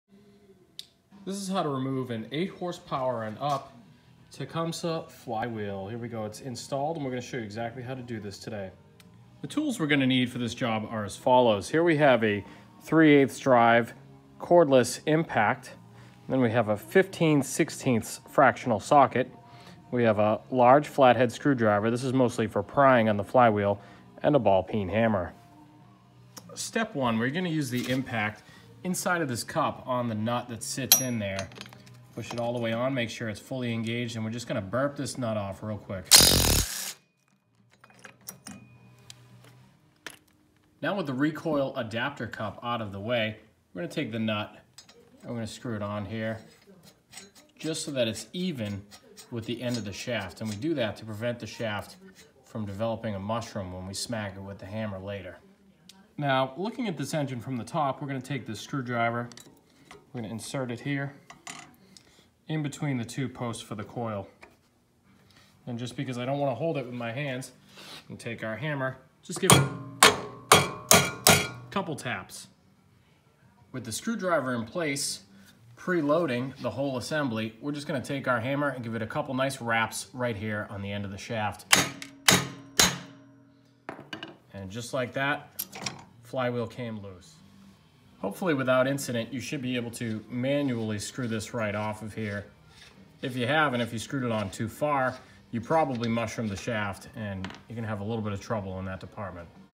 Tecumseh Horizontal Shaft 8-13 HP Flywheel removal
A brief video provided by bay state engines showing how to remove the flywheel from the crankshaft taper under the blower housing. By viewing this vid